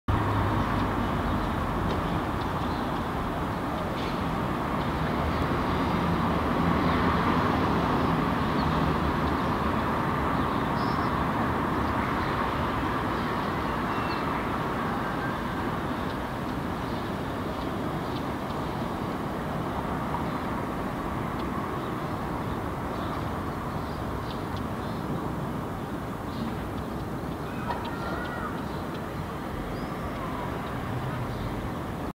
Wheatear fledglings on the rock slope
Three Northern Wheatear (Oenanthe oenanthe) fledglings together on the rock slope.

Kolme kivitaskun (Oenanthe oenanthe) maastopoikasta yhdessä kalliorinteellä.